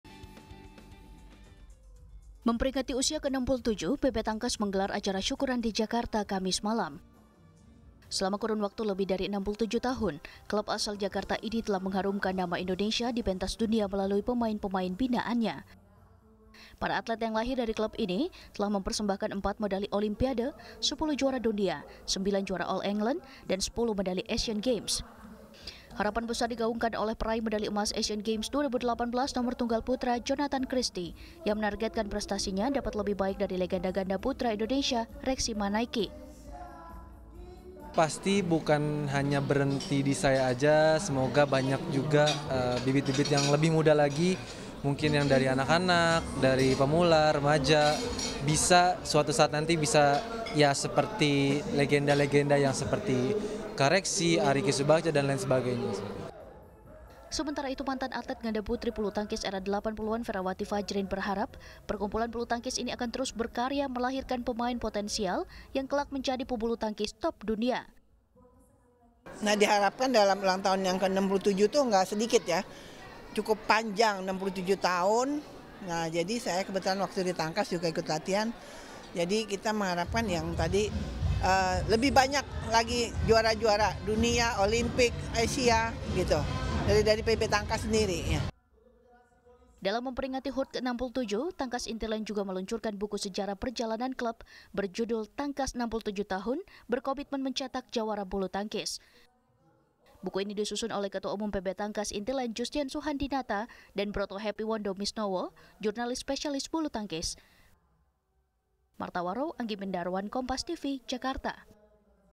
PB Tangkas Terus Bertekad Lahirkan Atlet Kelas Dunia
Perkumpulan Bulutangkis PB Tangkas Intiland merayakan hari jadi ke-67.

Di usia yang matang klub asal Jakarta ini bertekad melahirkan pebulu tangkis kelas dunia yang akan menjadi tulang punggung Indonesia di berbagai kejuaraan.

&nbsp;

&nbsp;